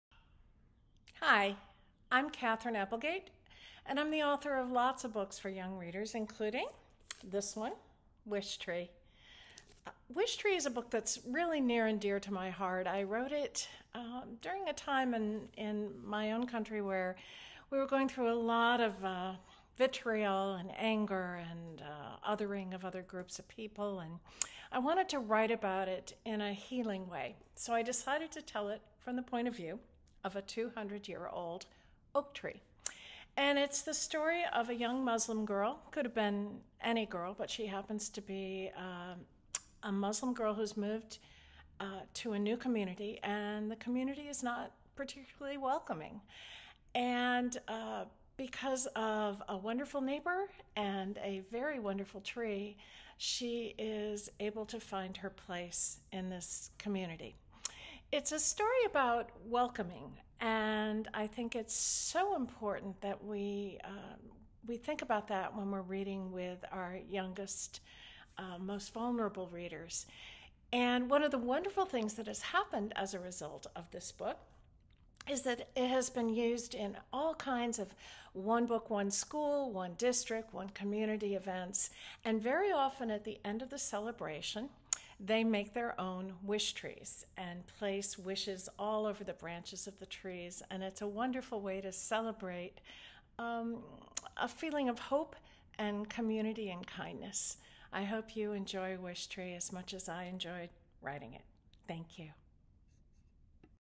Introducing Wishtree by Katherine Applegate
Newbery Medalist author Katherine Applegate introduces her New York Times-bestselling middle-grade novel about an oak tree and a crow who  help their neighbours embrace their differences.

Red is an oak tree who is many rings old. Red is the neighbourhood wishtree - people write their wishes on pieces of cloth and tie them to Red's branches. Along with a crow named Bongo and other animals who seek refuge in Red's hollows, this wishtree watches over the neighbourhood.

You might say Red has seen it all.

Until a new family moves in. Not everyone is welcoming, and Red's experience as a wishtree is more important than ever.
Funny, deep, warm, and nuanced, this is Katherine Applegate at her very best - writing from the heart, and from a completely unexpected point of view.

'The simplicity of Newbery Medalist Applegate's graceful novel contrasts powerfully with the prejudice it confronts... It's a distinctive call for kindness, delivered by an unforgettable narrator.' Publishers Weekly